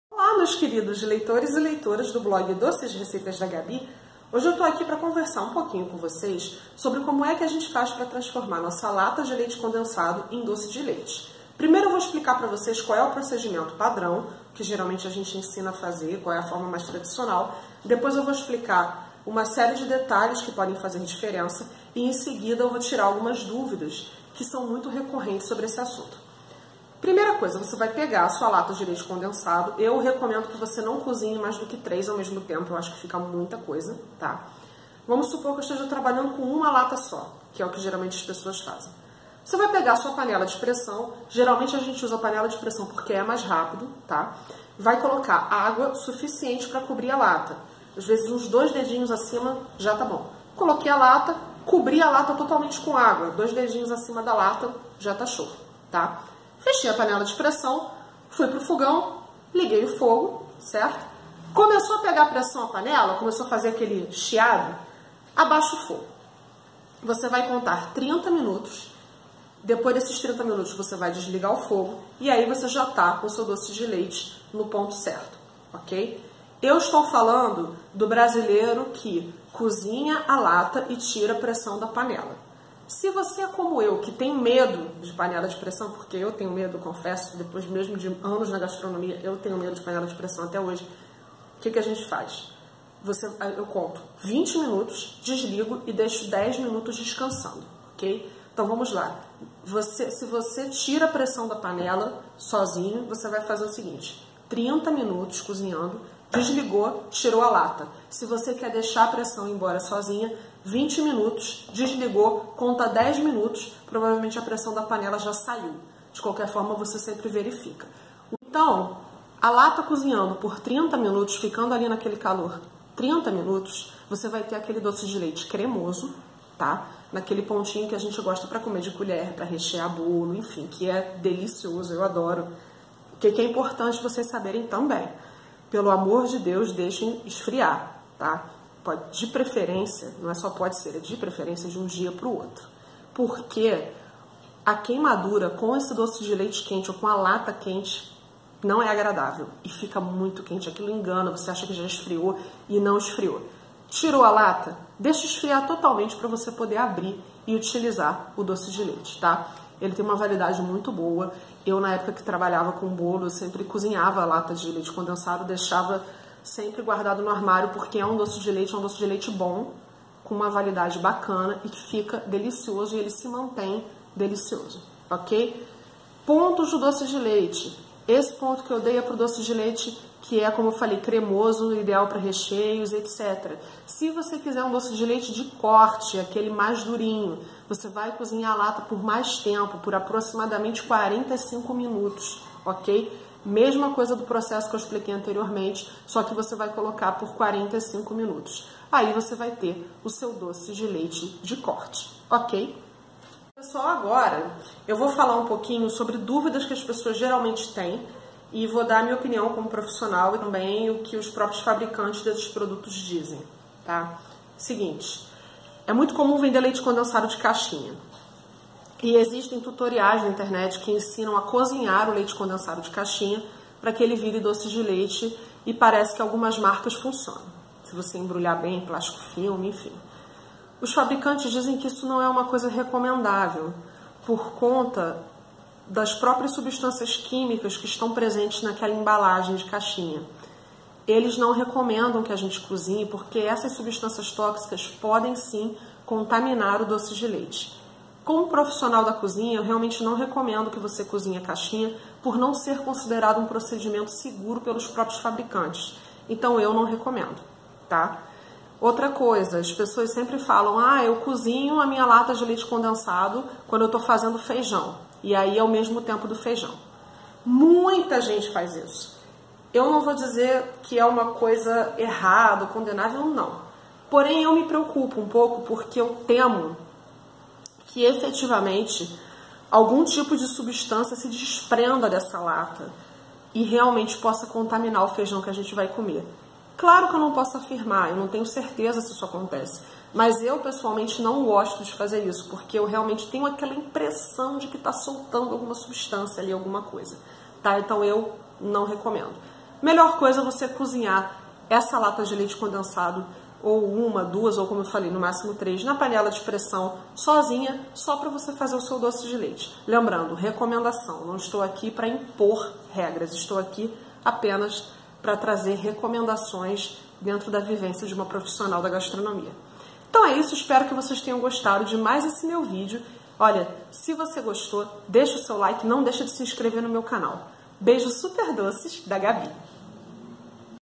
Dicas para cozinhar a lata de leite condensado
Dicas importantes para cozinhar a lata de leite condensado na panela de pressão, transformando-a num delicioso doce de leite.  Além de explicar o procedimento básico, eu respondo a dúvidas comuns como: É possível cozinhar a caixinha de leite condensado?

Beijos super doces da Gaby